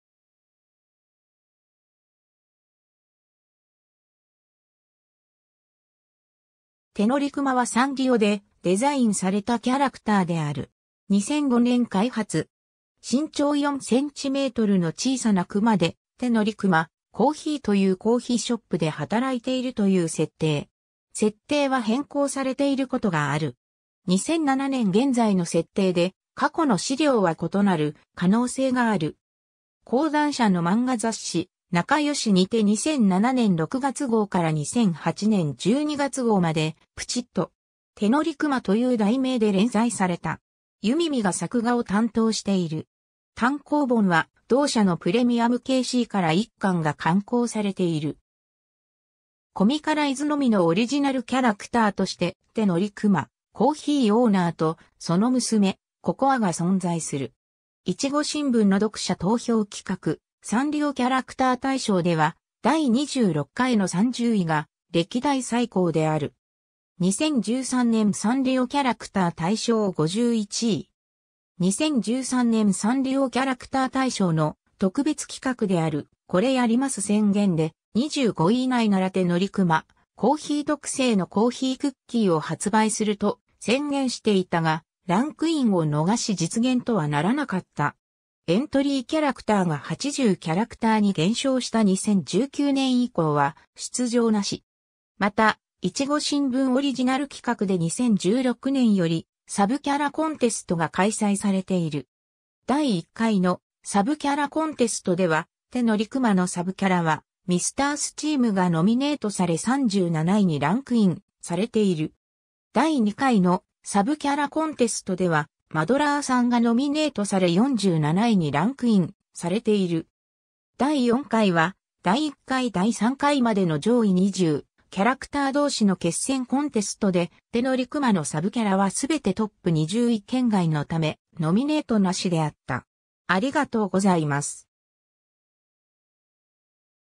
てのりくま
てのりくま（ラテン文字表記：TENORIKUMA）はサンリオでデザインされたキャラクターである。
2005年開発。
 身長4 cmの小さなクマで、「てのりくまコーヒー」というコーヒーショップで働いているという設定。
 ※設定は変更されていることがある。
2007年現在の設定で、過去の資料は異なる可能性がある。
 講談社の漫画雑誌『なかよし』にて2007年6月号から2008年12月号まで『プチっと!てのりくま』という題名で連載された。
ゆみみが作画を担当している。
単行本は、同社のプレミアムKCから1巻が刊行されている。
 コミカライズのみのオリジナルキャラクターとして、「てのりくまコーヒー」オーナーと、その娘・ここあが存在する。
 『いちご新聞』の読者投票企画「サンリオキャラクター大賞」では第26回（2011年）の30位が歴代最高である。
 2013年サンリオキャラクター大賞51位。
2013年サンリオキャラクター大賞の特別企画である「これやります宣言」で、「25位以内ならてのりくまコーヒー特製のコーヒークッキーを発売する」と宣言していたが、ランクインを逃し実現とはならなかった。
 エントリーキャラクターが80キャラクターに減少した2019年以降は出場なし。
 また、いちご新聞オリジナル企画で2016年より「サブキャラコンテスト」が開催されている。
第1回（2016年）の「サブキャラコンテスト」では「てのりくま」のサブキャラは「ミスター・スチーム」がノミネートされ37位にランクインされている。
第2回（2017年）の「サブキャラコンテスト」では「マドラーさん」がノミネートされ47位にランクインされている。
第4回（2019年）は第1回（2016年）〜第3回（2018年）までの上位20キャラクター同士の決戦コンテストで、「てのりくま」のサブキャラは全てトップ20位圏外のためノミネートなしであった。